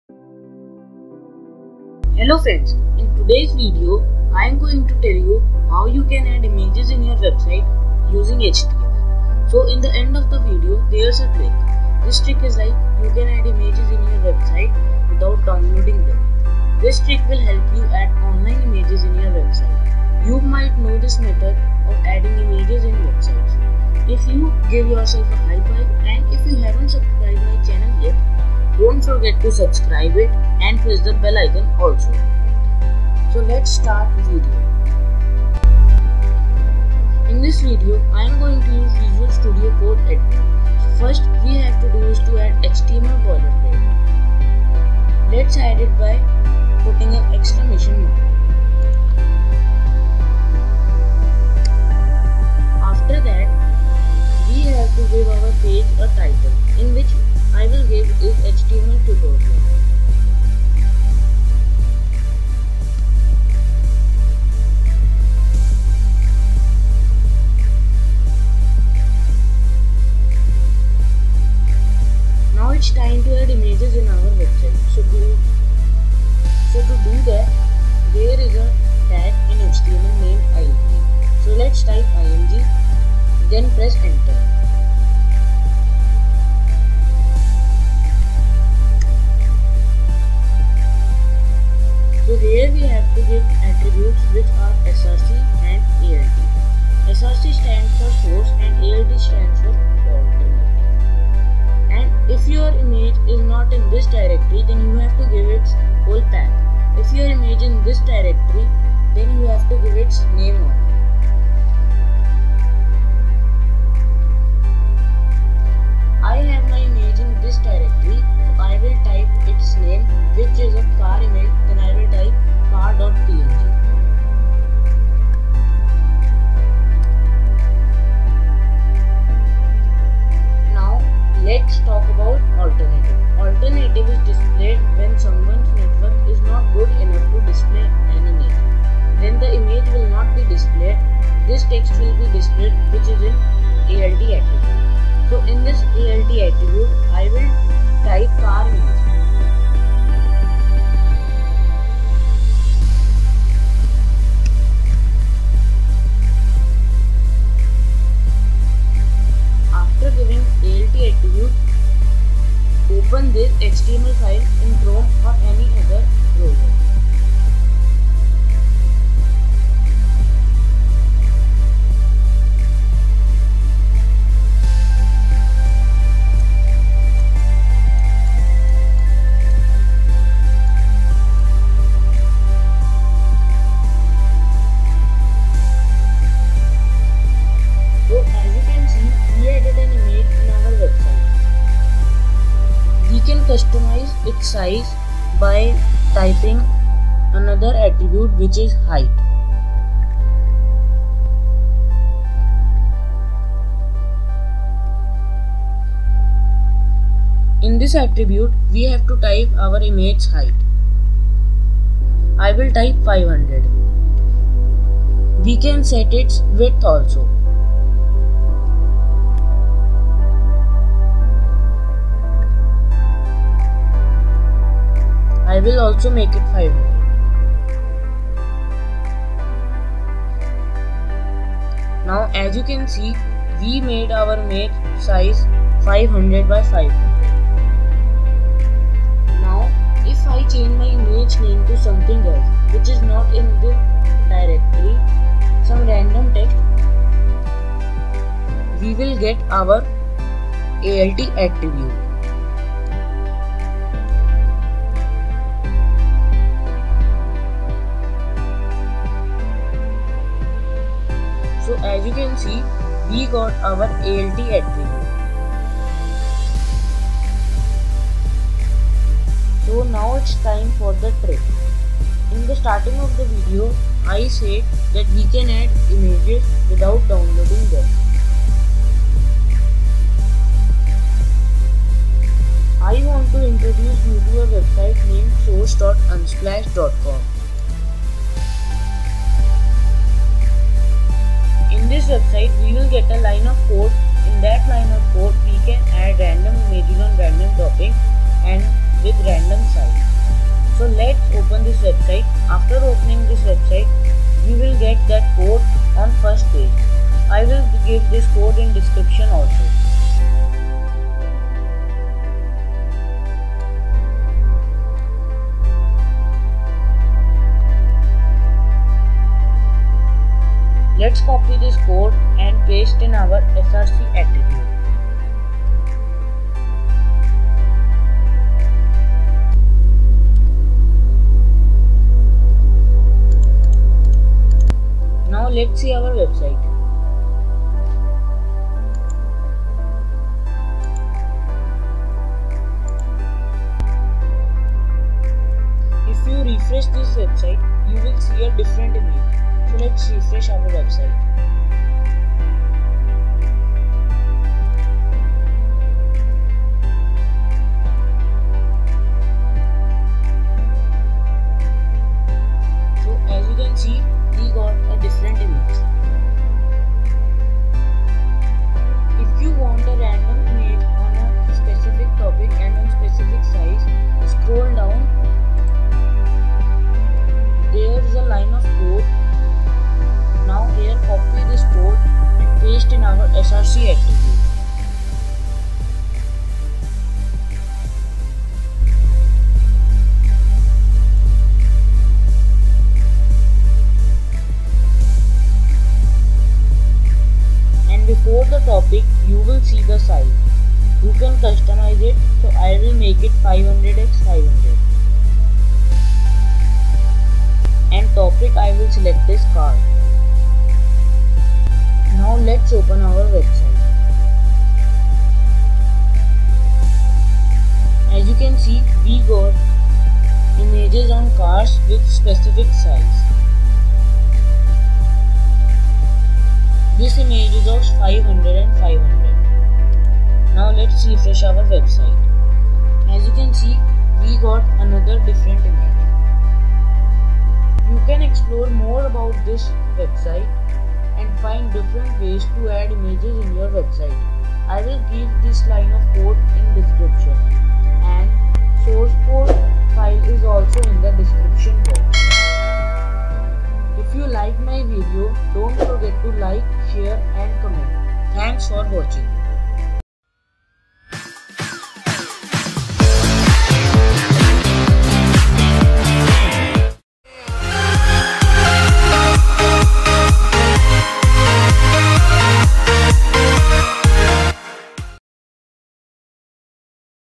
ADD IMAGES IN YOUR WEBSITES USING HTML | BONUS TRICK | UNSPLASH SOURCE | PROGRAMMING HERO
HELLO PROGRAMMERS

To add random images in your websites paste this code in src attribute
To add random image on specific topic and size (type the topic on which you want the images at the end of this code) :